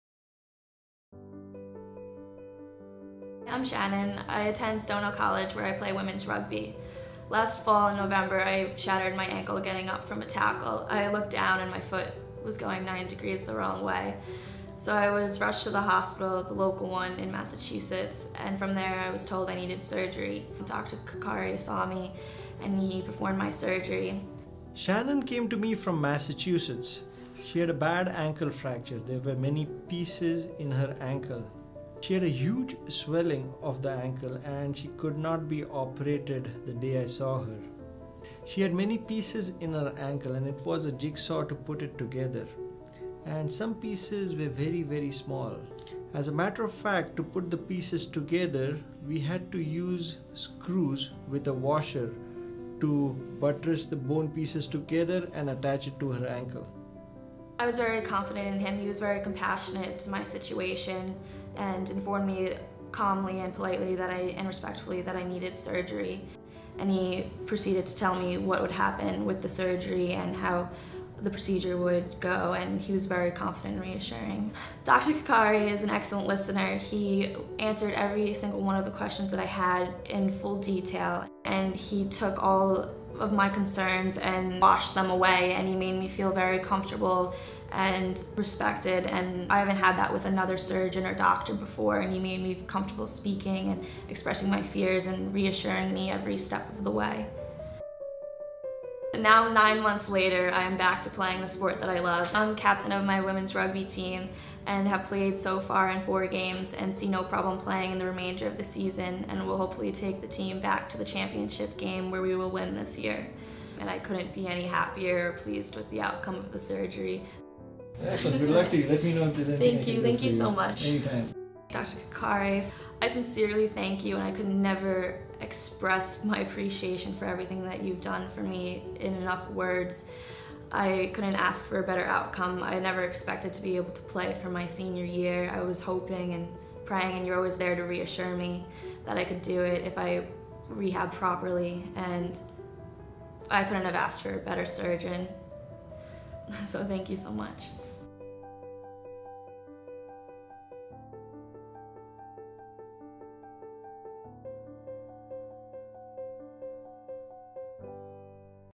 Rugby injury- multiple fractures- surgical fixation Dr. Nakul Karkare
Ankle fractures are relatively common injuries which involves the distal tibia, distal fibula, talus, and calcaneus.

to find out more